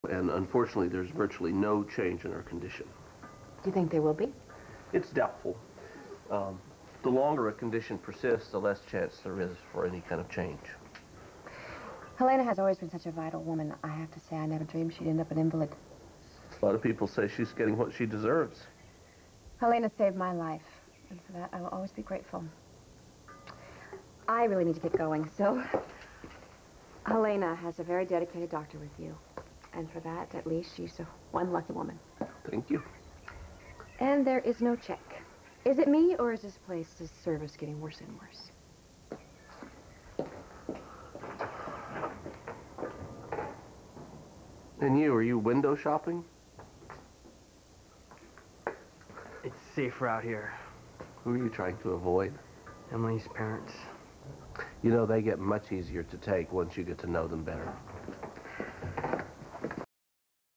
Tony advises Katherine and Juan
1999 - Tony gives Katherine his opinion on Helena's medical condition, then
teases Juan for avoiding the Qs.